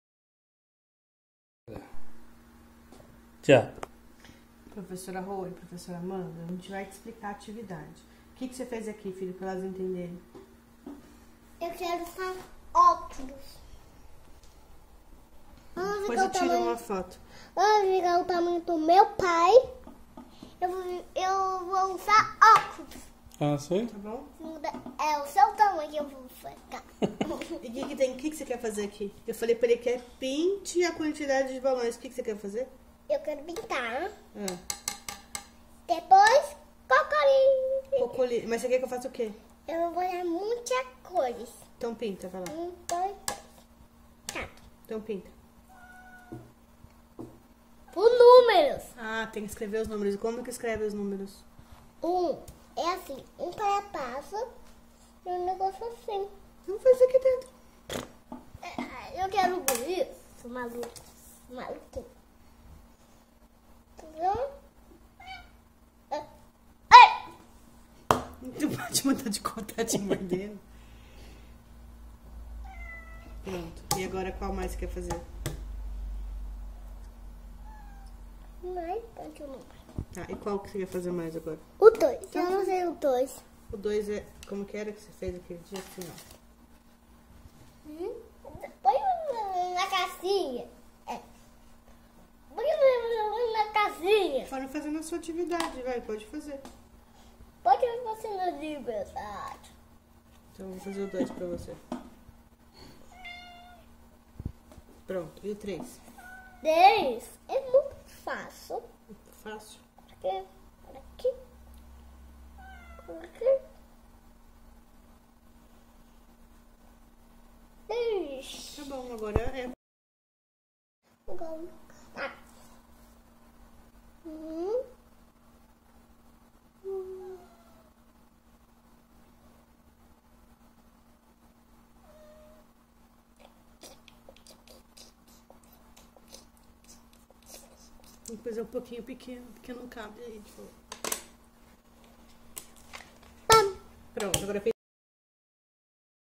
Tarefas da escola
A Miguel le dan pocas tarea en la escuela.
A Miguel tem poucas tarefas na escola.
así es como las hace.